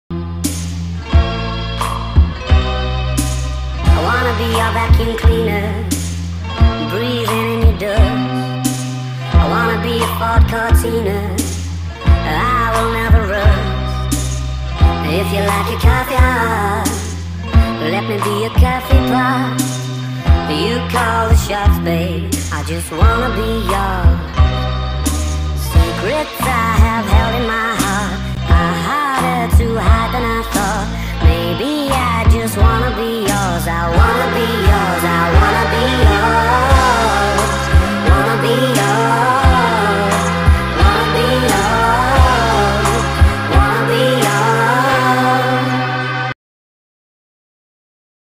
My favorite yugioh ships but boy x boy
please don't kill me it's my opinon and i know yami marik and yami bakura were not good to ryo and malik but if this two wouldn't be evil i think they would be cute and the sexuality is just my headcanon and no yami yugi malik marik bakura and ryo are not the same person they just share there bodies and soul and if somone is homophobic i will delete it and you will not be allowed to look at my channel ever agian i also do stright ships but not yet